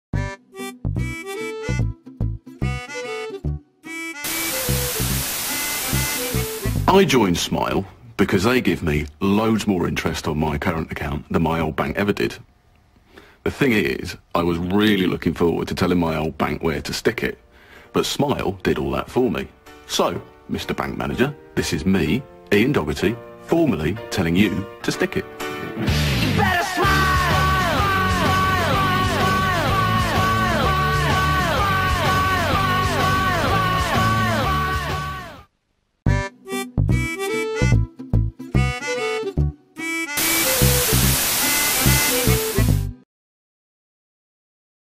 Smile.co.uk Advert - 2004
An advert for Smile.co.uk shown on Central (now ITV) in 2004.